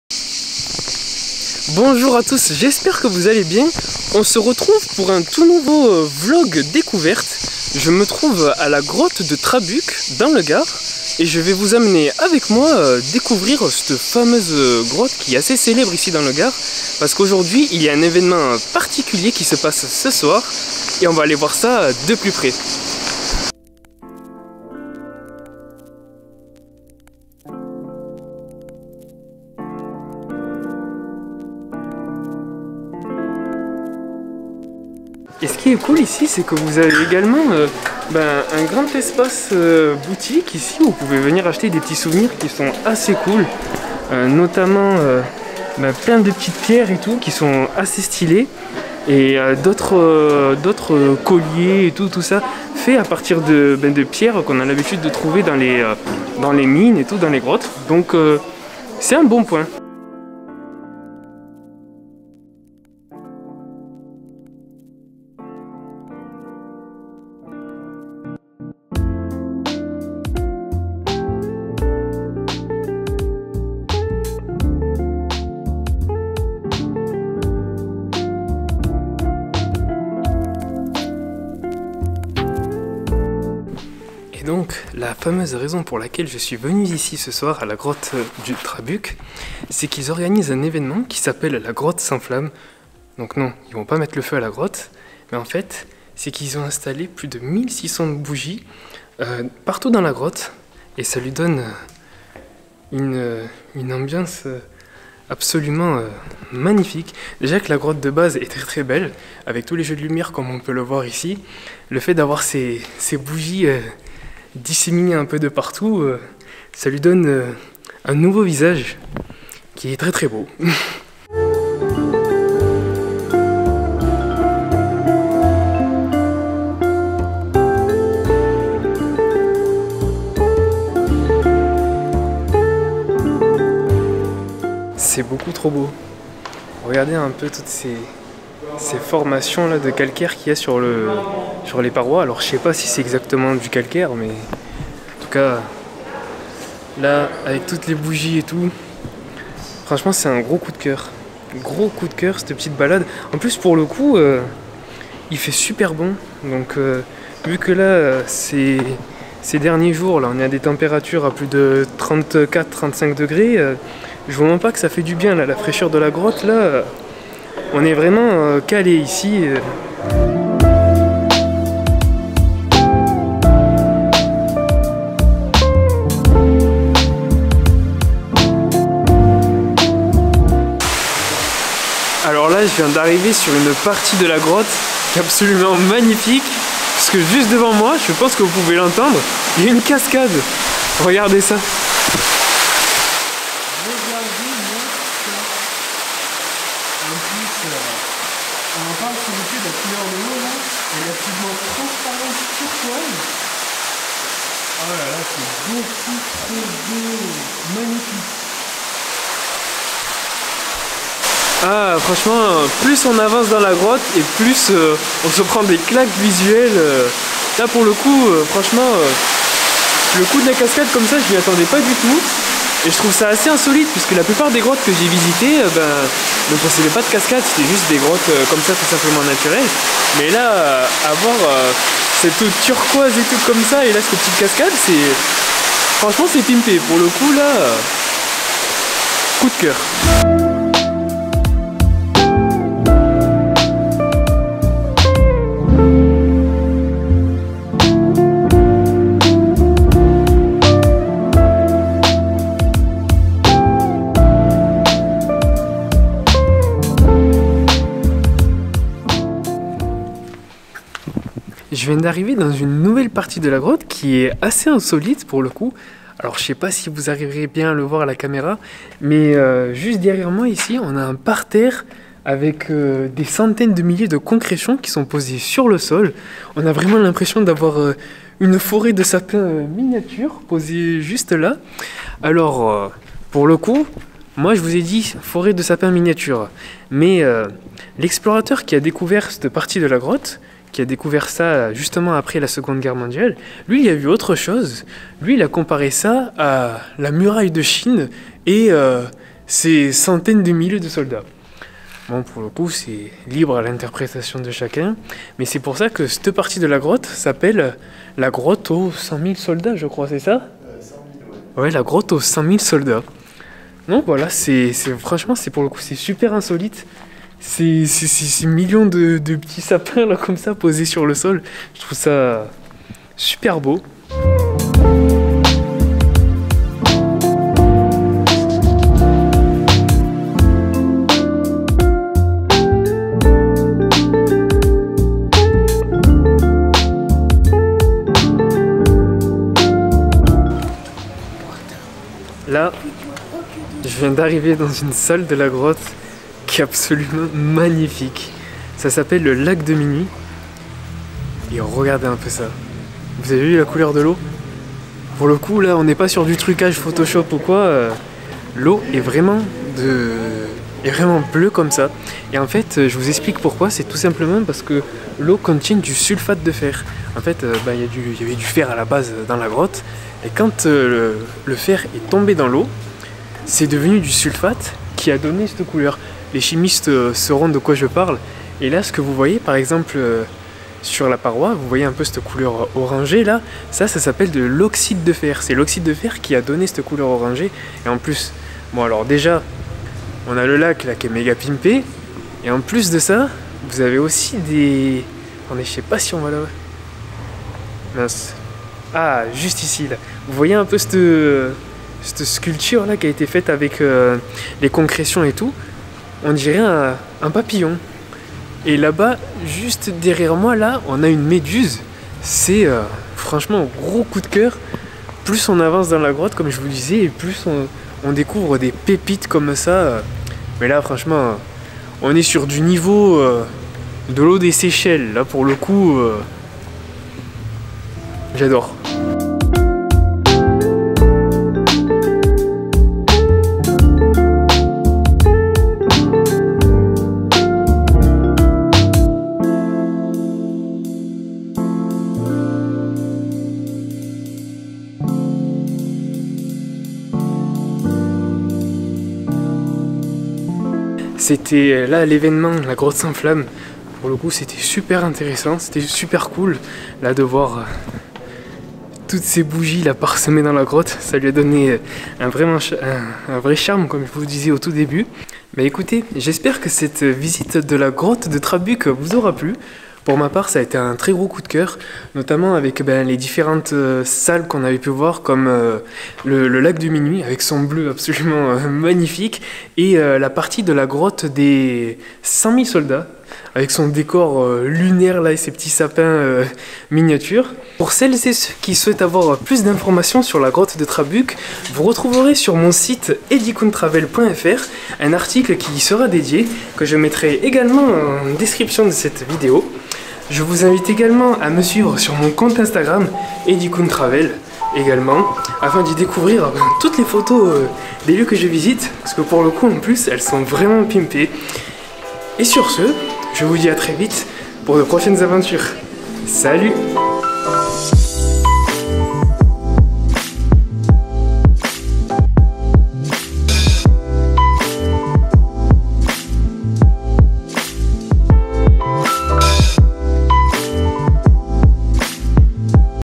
La Grotte De Trabuc - Visite Insolite dans le Gard (Vlog France)
Bienvenue dans ce tout nouveau vlog découverte ! Aujourd'hui je vous fais découvrir l'une des plus belles grottes naturelles du Gard : La Grotte de Trabuc. Je m'y suis rendu pour un événement particulier que la grotte organise uniquement l'été,  une visite insolite du nom de "la grotte s'enflamme" ! La Grotte est décorée avec plus de 1600 bougies, qui lui donne une ambiance incroyable !

Plus d'information sur la Grotte de Trabuc prochainement sur mon